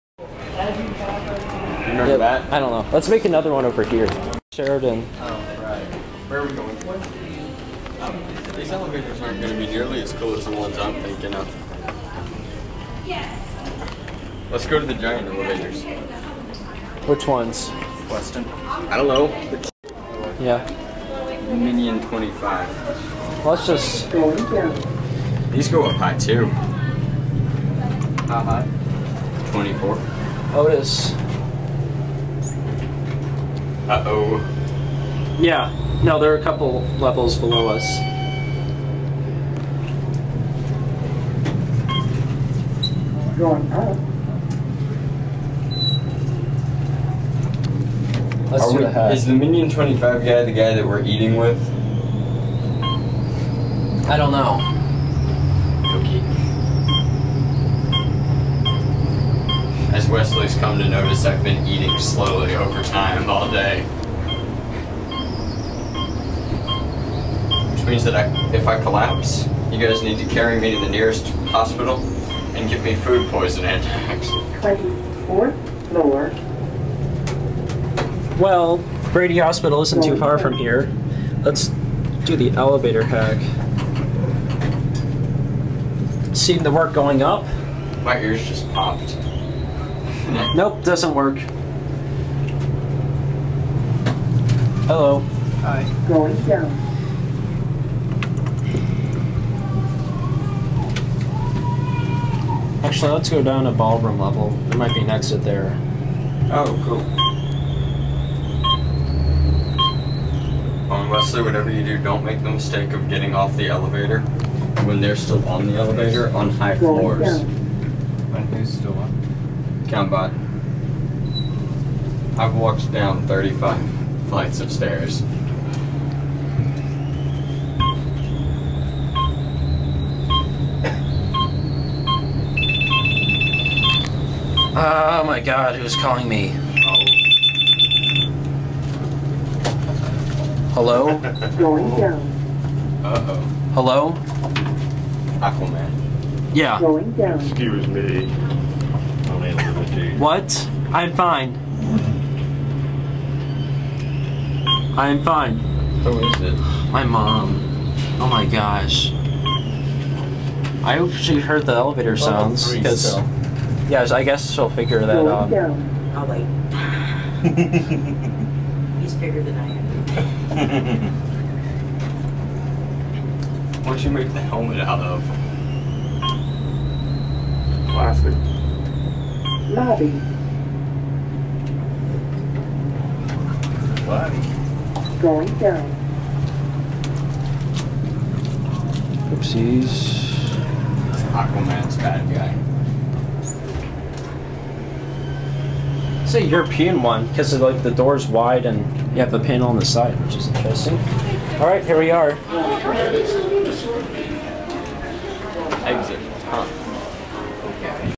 Otis Traction Elevators at Hyatt Regency Atlanta Int'l Tower
Riding the elevators at the Hyatt Regency during dragon*con 2008.